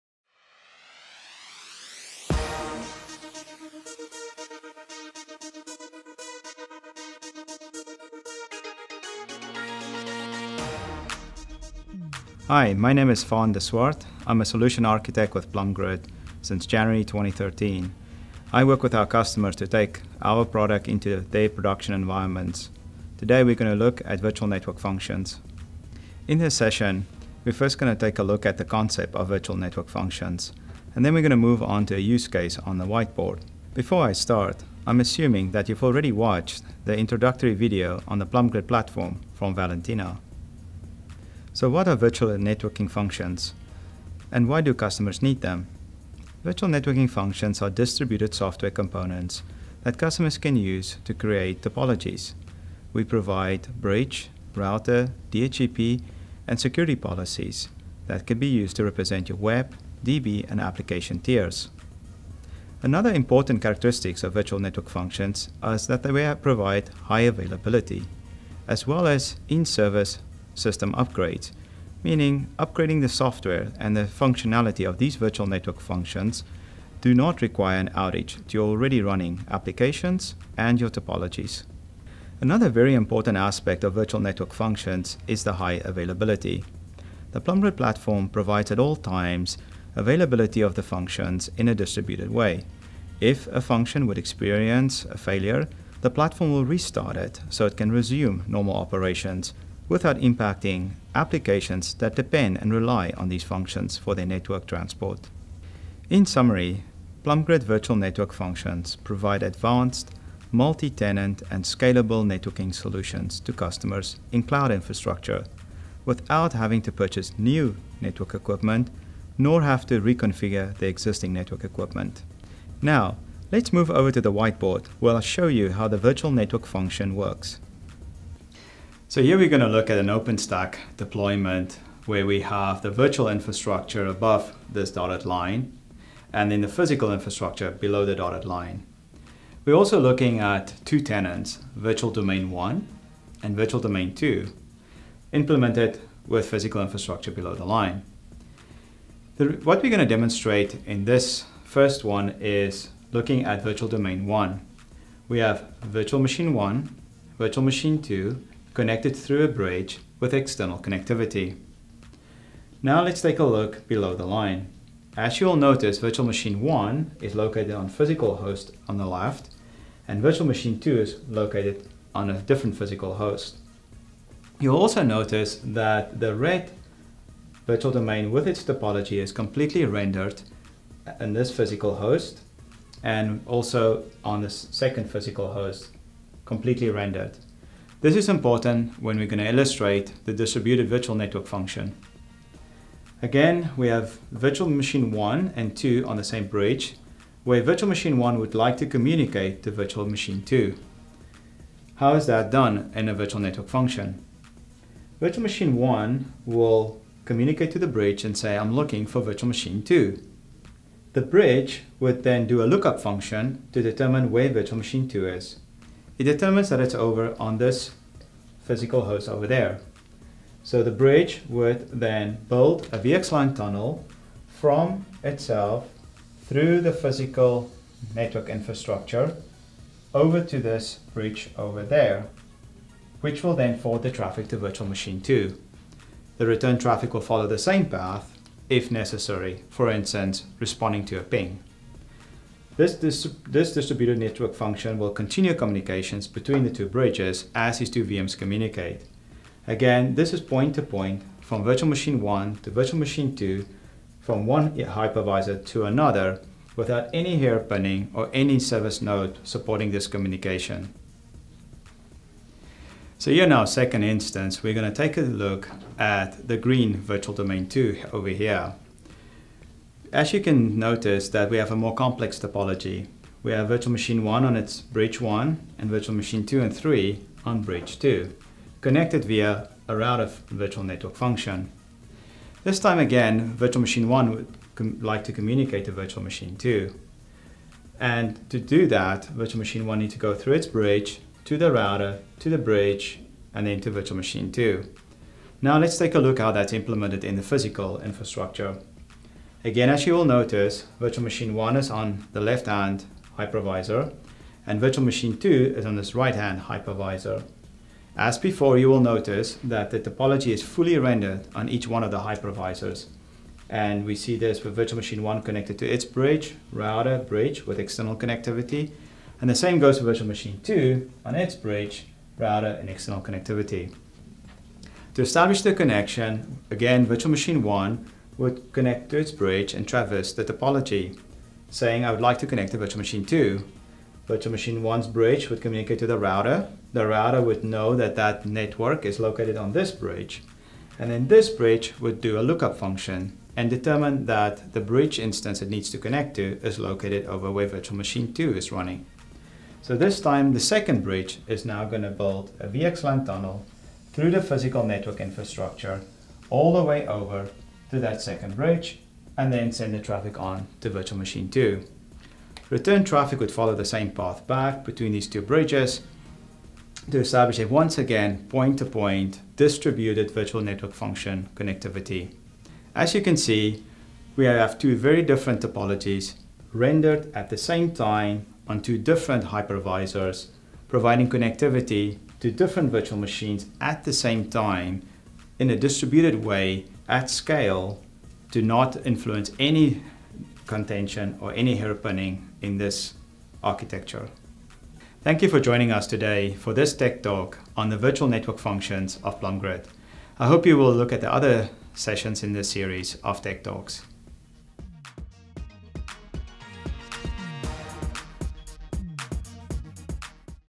PLUMgrid Distributed Virtual Network Functions
Pre-requisites: You have watched Red Chair Series PLUMgrid Platform and Virtual Domain videos

You’ll learn about rich set of Virtual Network Functions (VNFs) library which comes in PLUMgrid ONS. Video provides an overview about how to deploy complex virtual network topologies with VNFs in a Virtual Domain™. You’ll learn how easy it is to design and implement point to point connectivity tunnels with VNFs in a PLUMgrid zone using VXLAN standards.